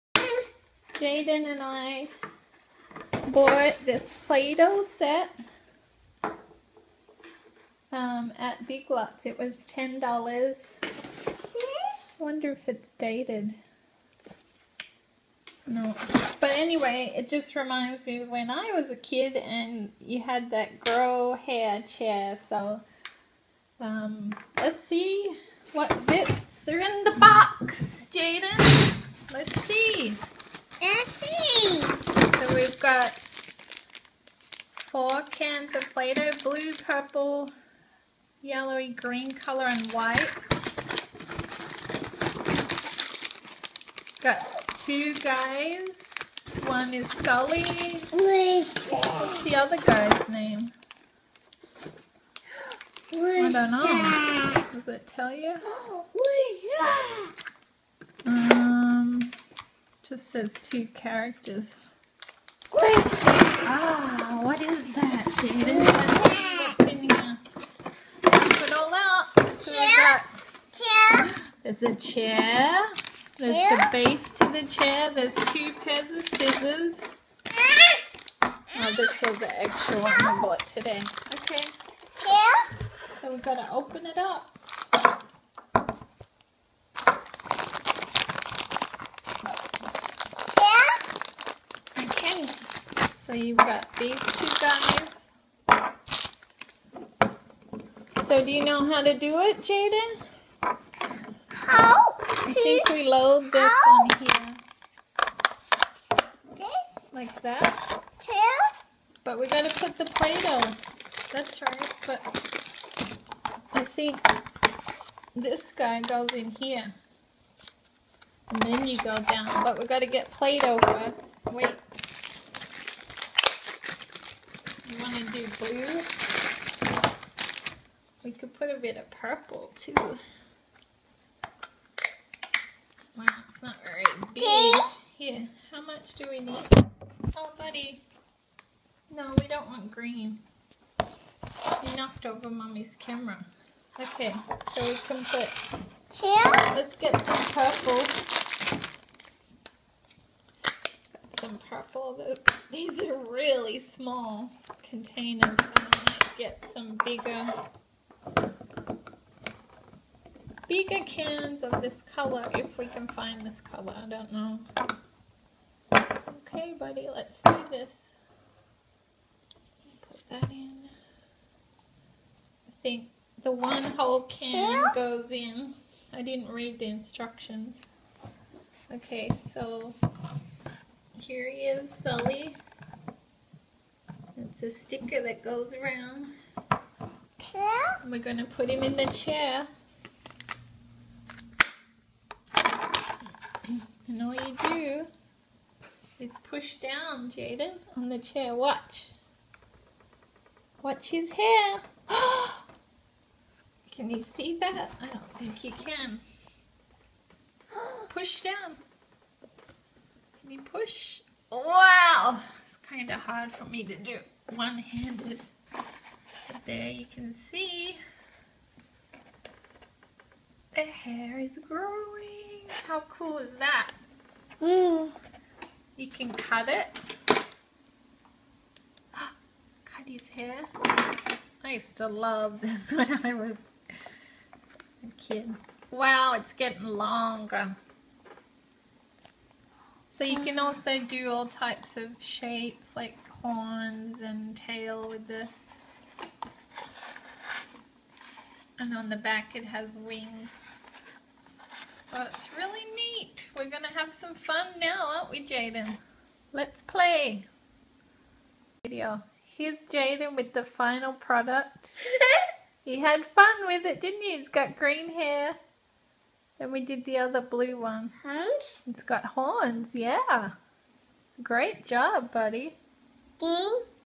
Monster University Scare Chair Playdoh Review
My Son and I are having a ball together playing with toys, isn't he the cutest?